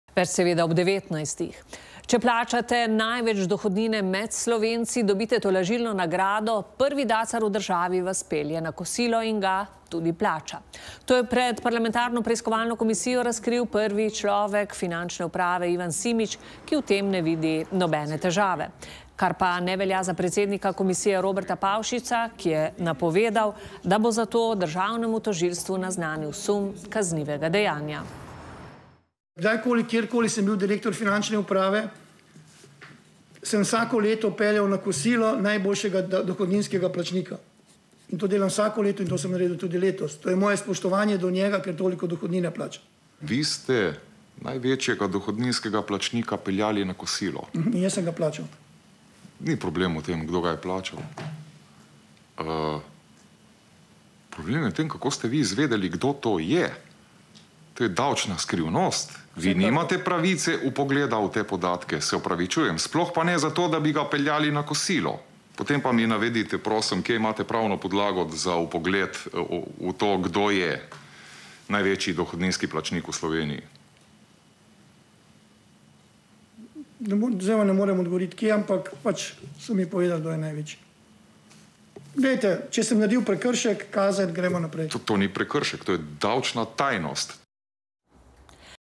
Ivan Simič - Povabilo na kosilo
The highest tax payer is invited to launch by director of financial administration Ivan Simič. It's against the law to gain info who that that person is. :D